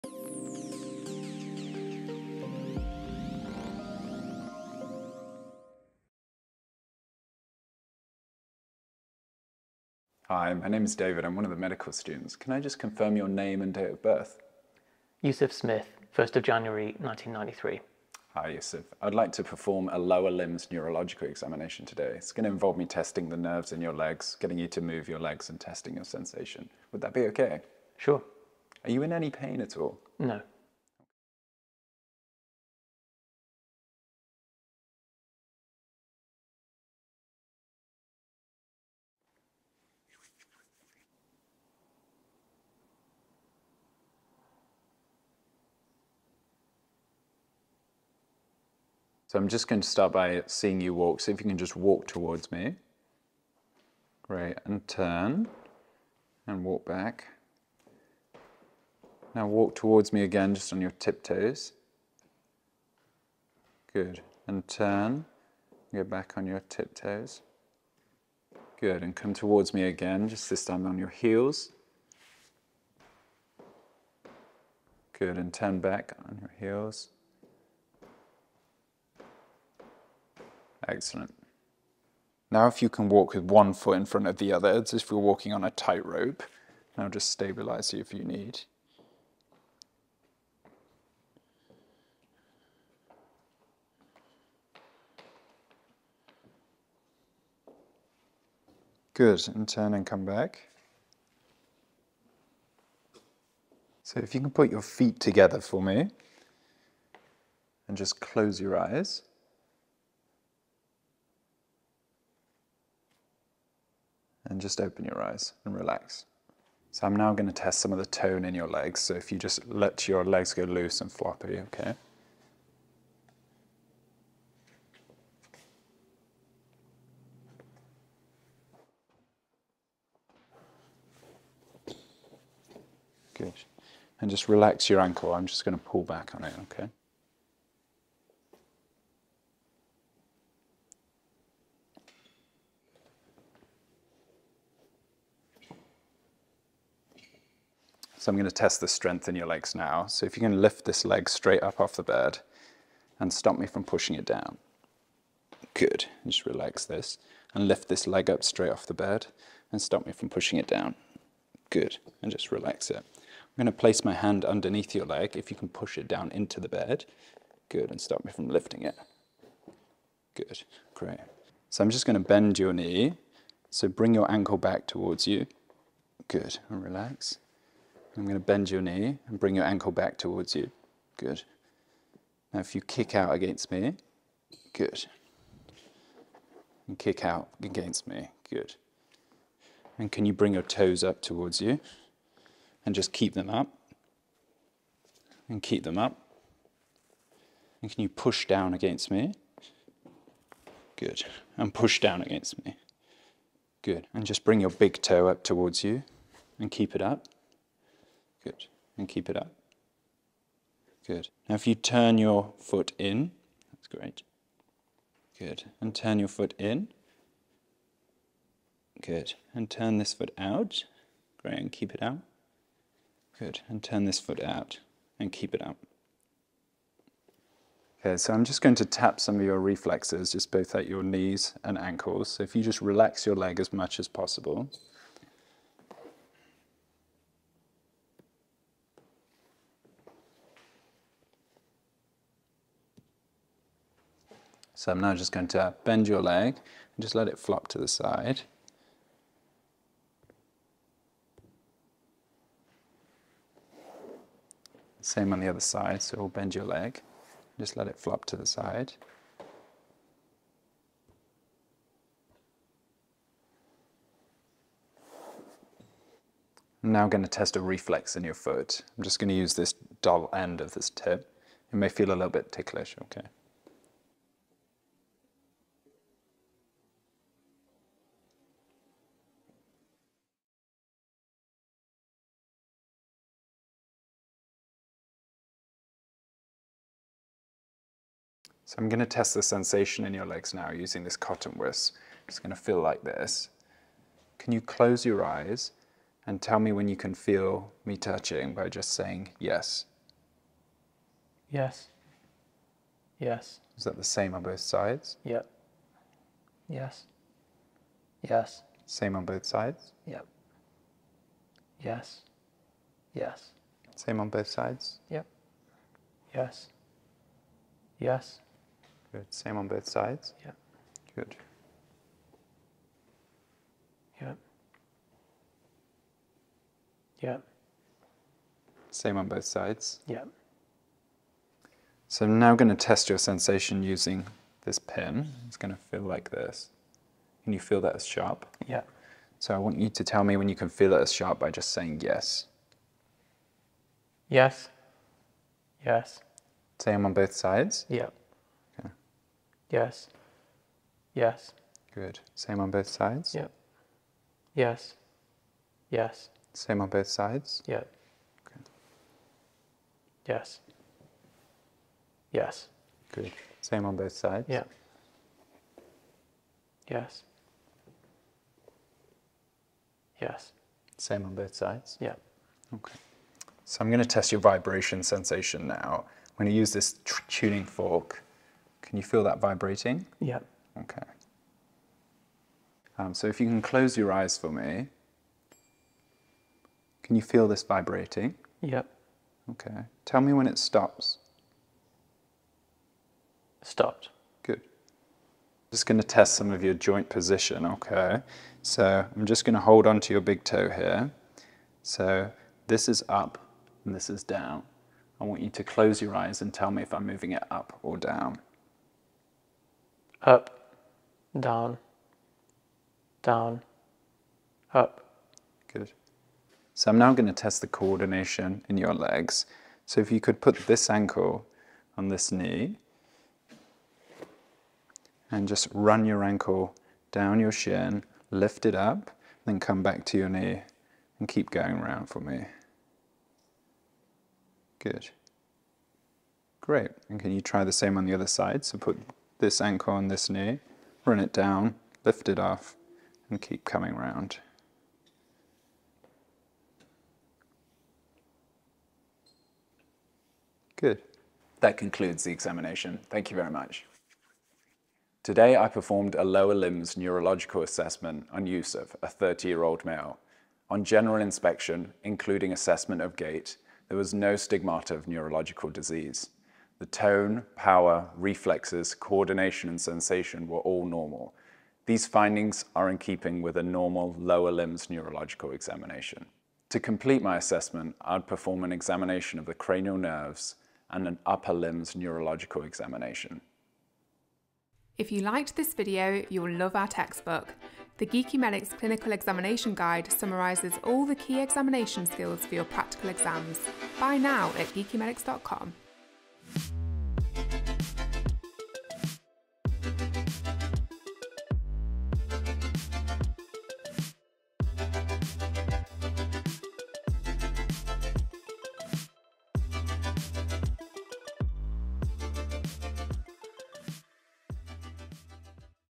Lower Limb Neurological Examination | OSCE Guide | NEW | UKMLA | CPSA | PLAB 2
This video provides a step-by-step demonstration of how to perform a lower limb neurological examination in an OSCE station, including the assessment of tone, power, reflexes, sensation and coordination.

Chapters:
- Introduction 00:00
- General inspection 00:40
- Gait 00:48
- Romberg's test 01:44
- Tone 01:55
- Power 02:28
- Reflexes 03:48
- Light touch sensation 04:47
- Pinprick sensation 05:31
- Vibration sensation 06:13
- Proprioception 06:35
- Co-ordination 06:56
- Summarise findings 07:34